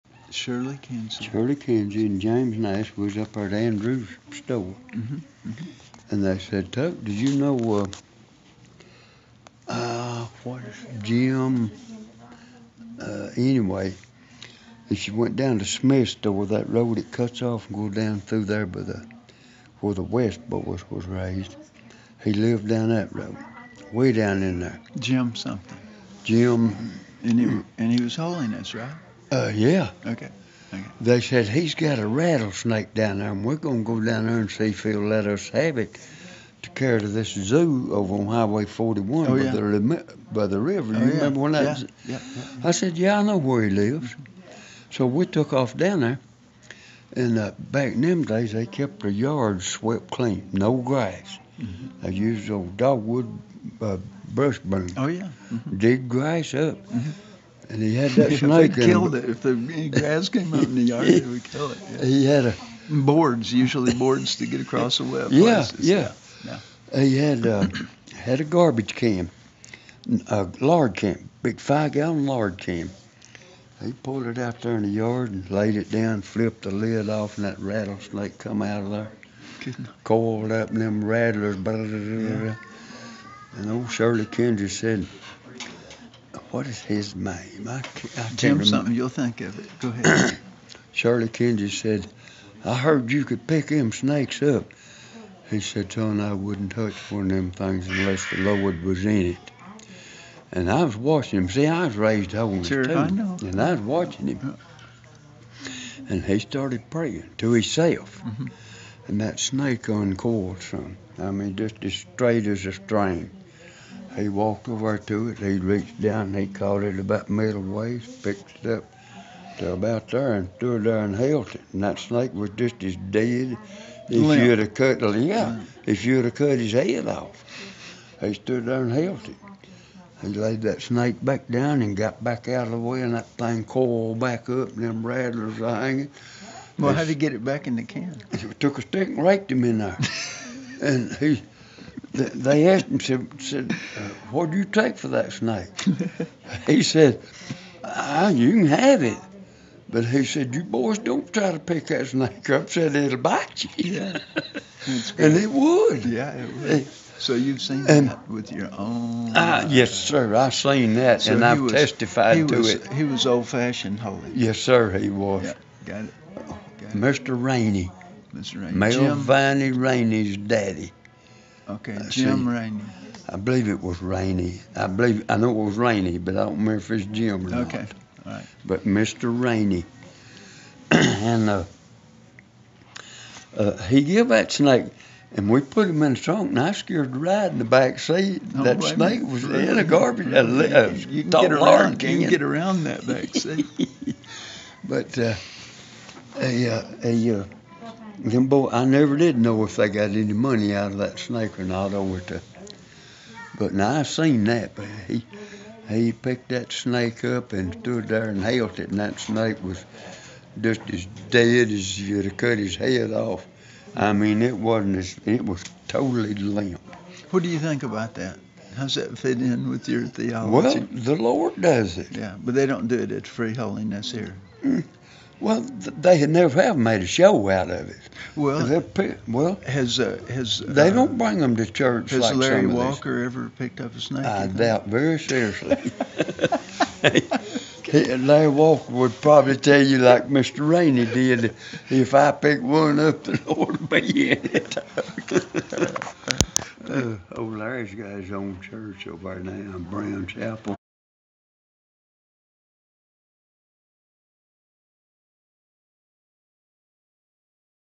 Uncle Tuck's Snake Story
My Uncle Tuck tells his true story.
Recorded at my Dad's funeral, June 2022.